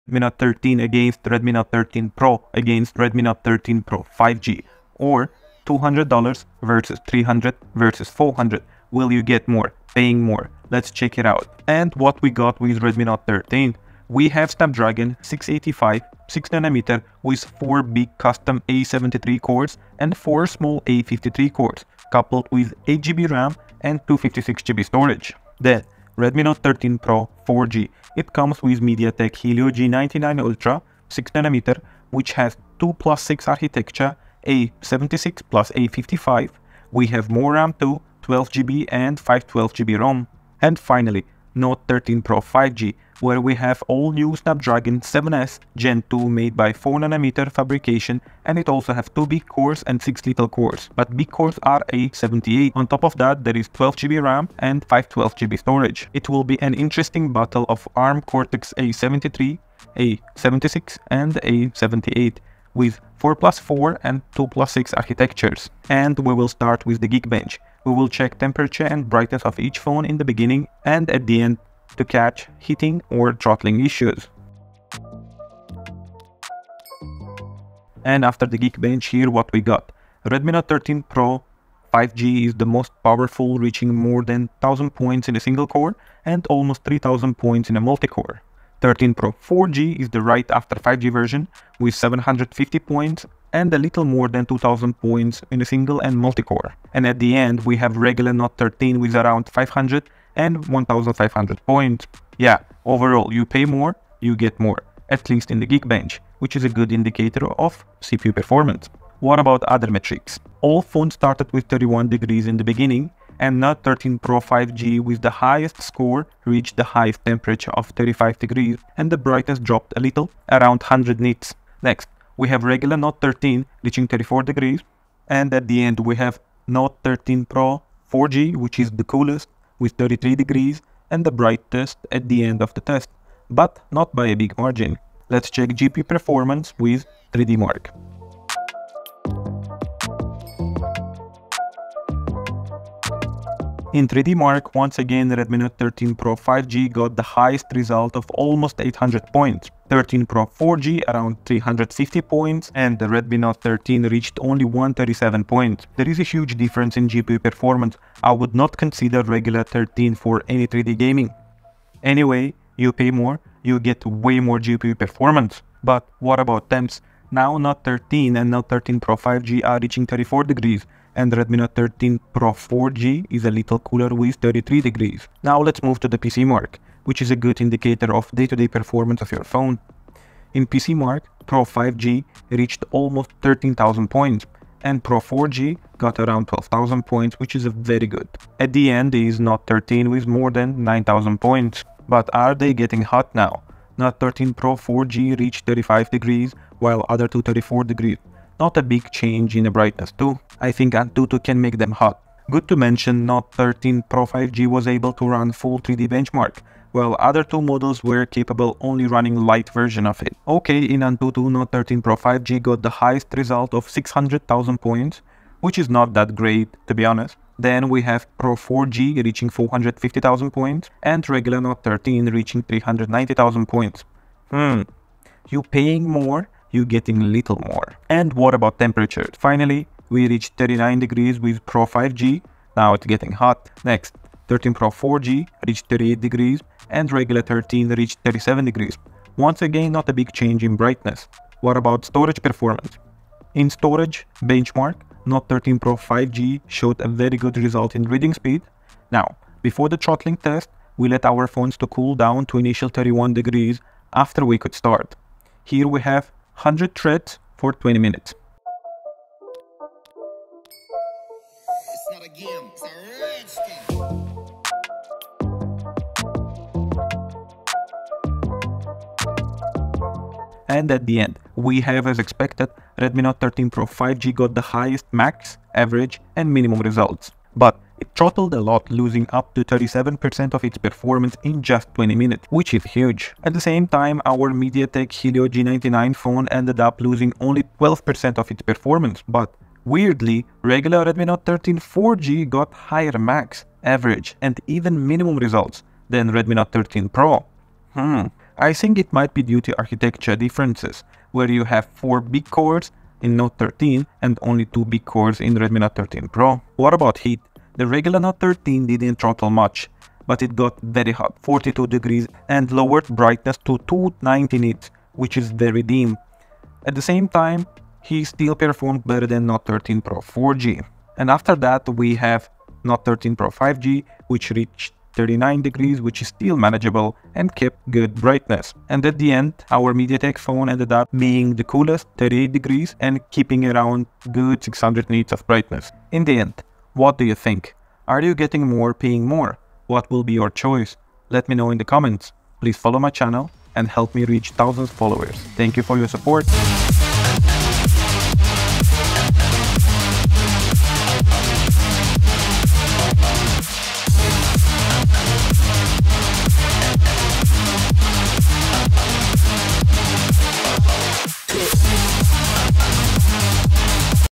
Redmi Note 13 vs Note 13 Pro vs Note 13 Pro 5G - AnTuTu, Throttling, Geekbench, 3Dmark, PCMark, Heat
Redmi Note 13 4G vs Redmi Note 13 Pro 4G vs Note 13 Pro 5G - AnTuTu, Throttling, Geekbench, 3Dmark, PCMark, Heating, Brightness, Storage, CPDT Test. Performance review and Performance Test, where we comparing Qualcoom Snapdragon 680 vs MediaTek Helio G99 Ultra vs Qualcomm Snapdragon 7s Gen 2.